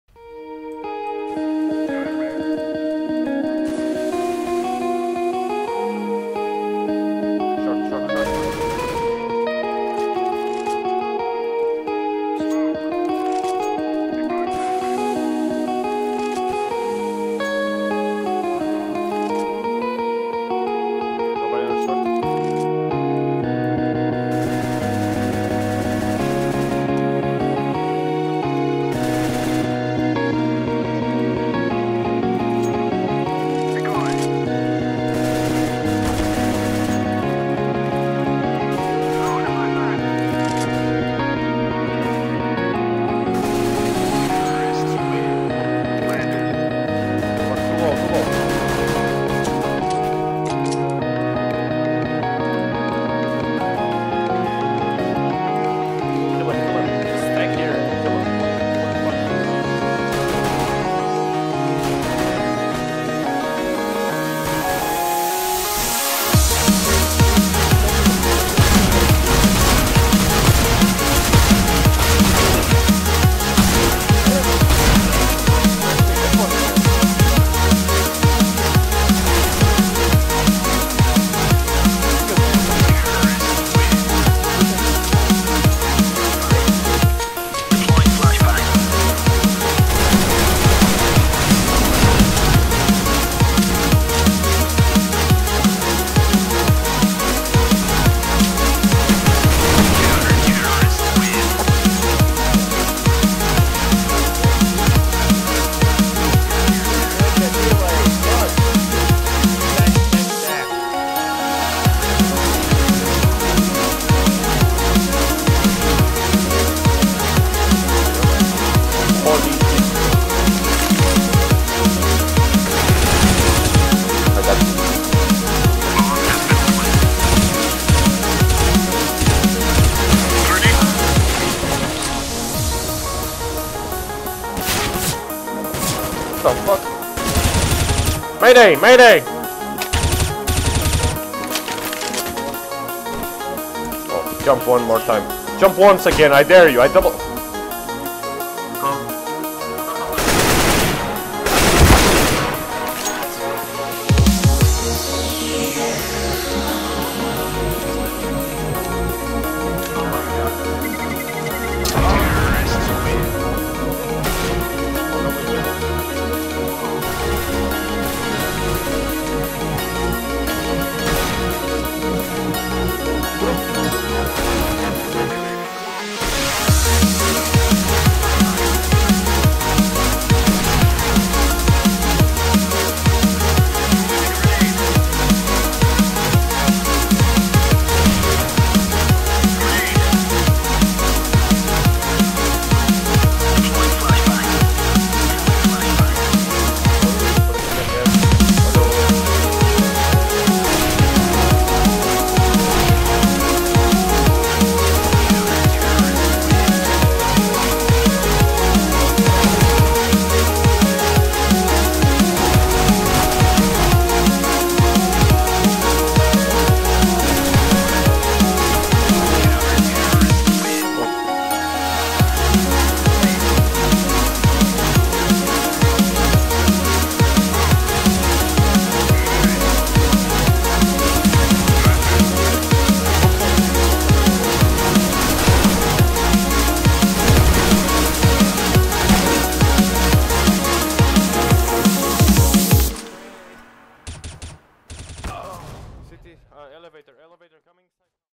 Soul'R Highlights Match Making and CEVO :)
Couple of highlights playing match making and cevo,

Mouse Specs: Steelseries Rival, 400 dpi, 1.7 Sense ingame and 6/11 windows.